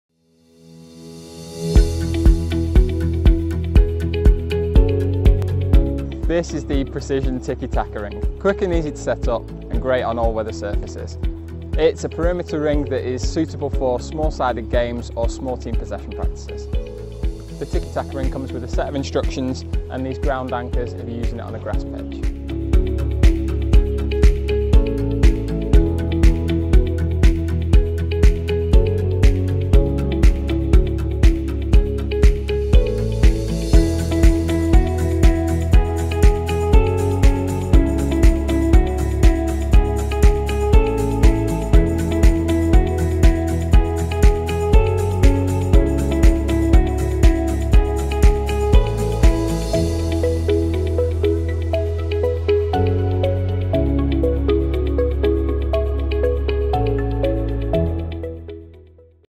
Precision Tiki Taka Ring Product Video
Precision Tiki Taka/Rondo Ring 17ft tiki taka ring is a football training tool for small sided passing games. The ring is designed to improve passing abilities and composure under pressure. Ideal for small sided possession games. Suitable for all age groups